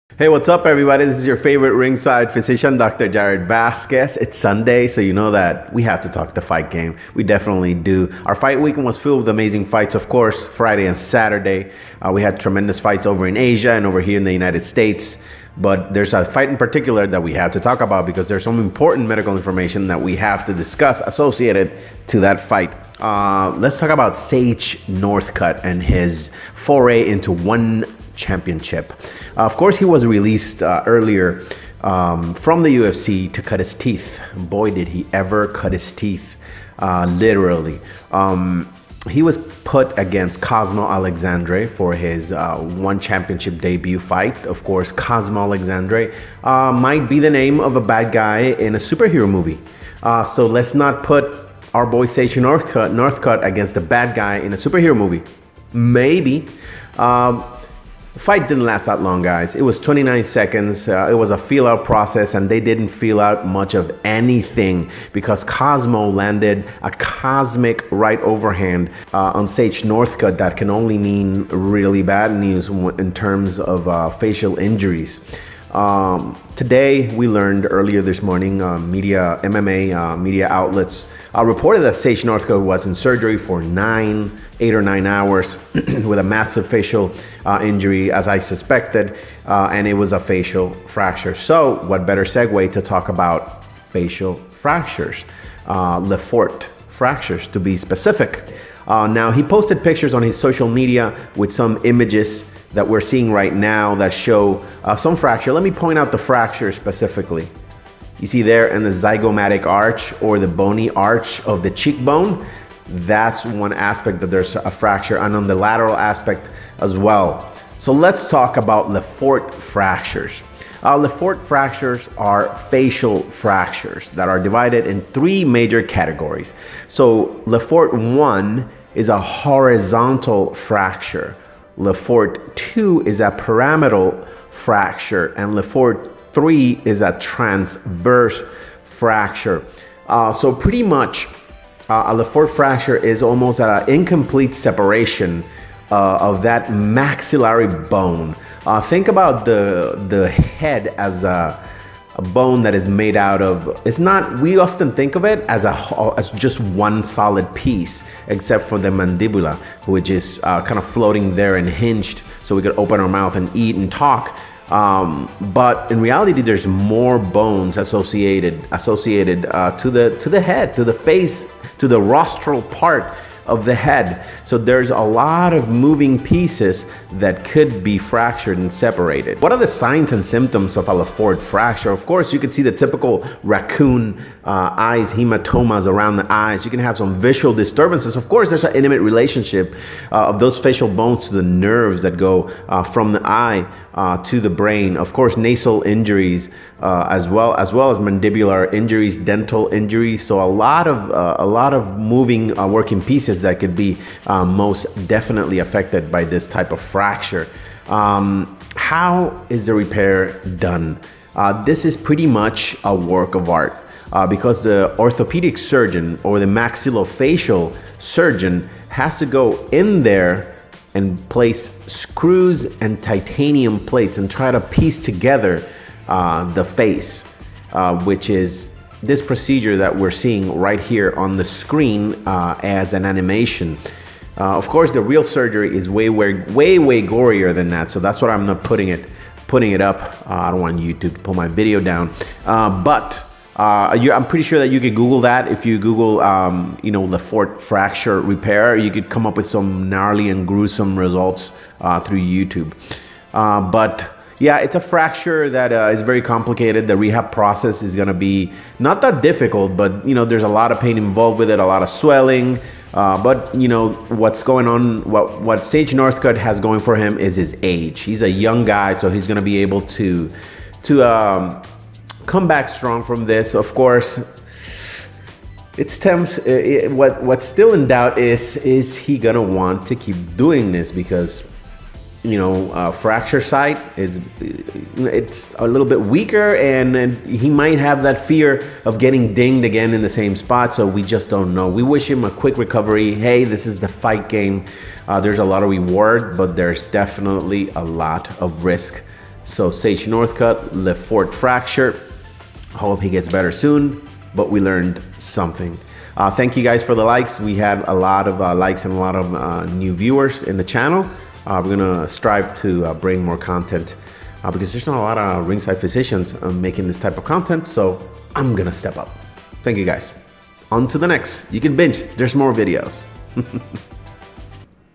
Sage Northcutt Facial Fracture
Sage Northcutt’s first career knockout loss came at a costly price.

Northcutt’s anticipated ONE Championship debut went down Friday in Singapore, and it was essentially a worst-case scenario. Northcutt (11-3) was knocked out in brutal fashion just 29 seconds into his fight with Cosmo Alexandre (8-1), getting left face first on the canvas after taking a huge punch.

Although Northcutt was alert and walking around the ring following the knockout, the damage was extensive. He shared an update from the hospital on Saturday night and revealed that he’d undergone a nine-hour surgery for eight facial fractures sustained in the contest.